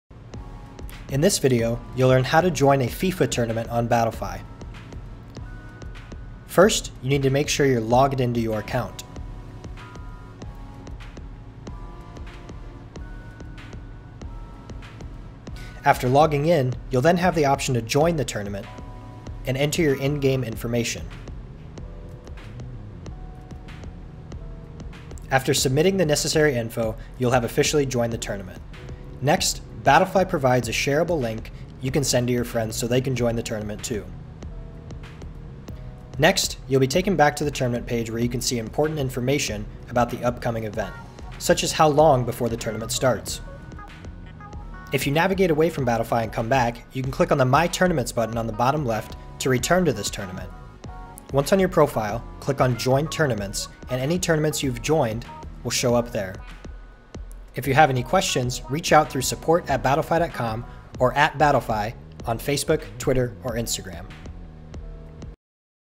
How to join a FIFA tournament on Battlefy | 2019
Learn how to join a FIFA tournament on Battlefy so you can play against the competition for cash and prizes

Battlefy is the easiest way to create, manage, and find esports tournaments.